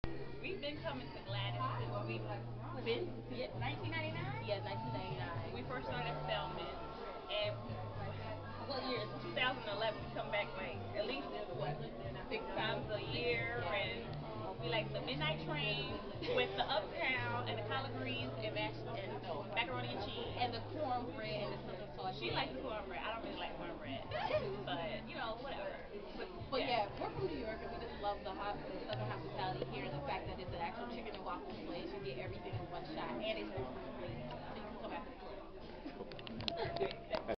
Madea's Restaurant Wars at Gladys Knight and Ron Winans Chicken and Waffles In Atlanta
Show your support for Gladys Knight and Ron Winans Chicken and Waffles for best restaurant in Atlanta